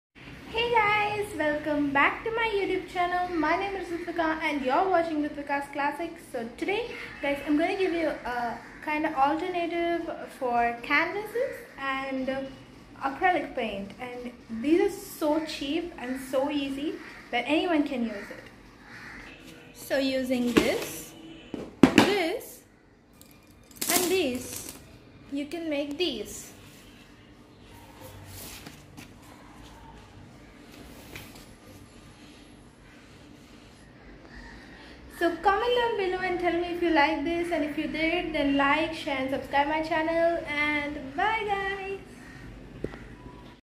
Use this instead of canvas if you don't have a one
Hey guys
In this video I gave an alternative for canvas and acrylics paint.. But this is only my opinion.
The chart paper nearly was close enough to canvas..